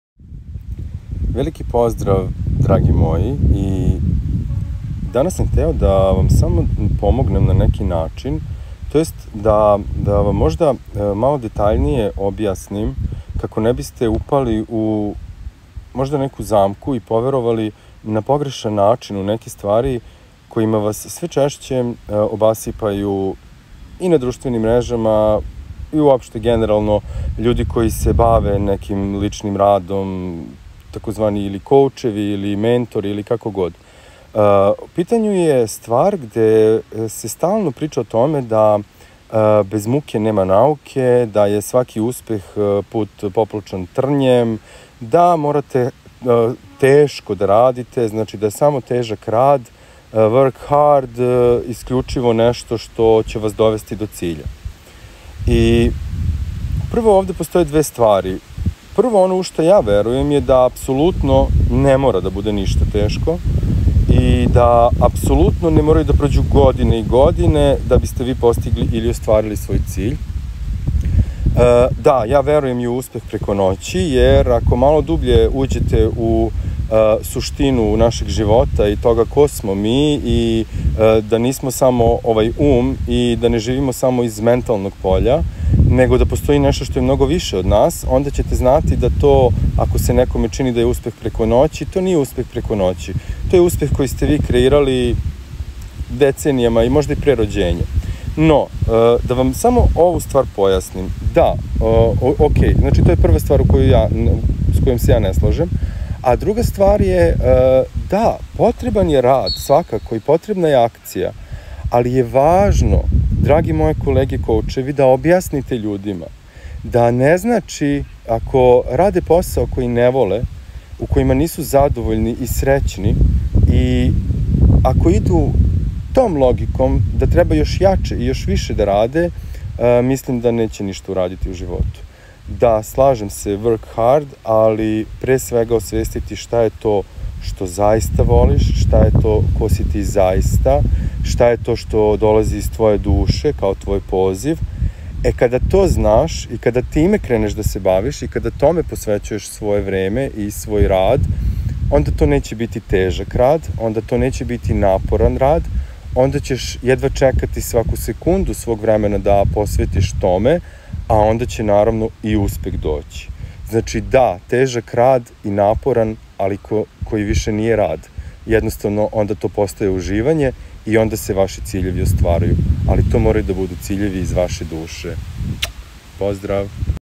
STOP MUKAMA! NAJVAŽNIJI SAVET DA ŽIVITE LAKOĆU! - Dr Nenad Petković
Život ne mora i ne treba da bude težak! Nije tako zamišljen!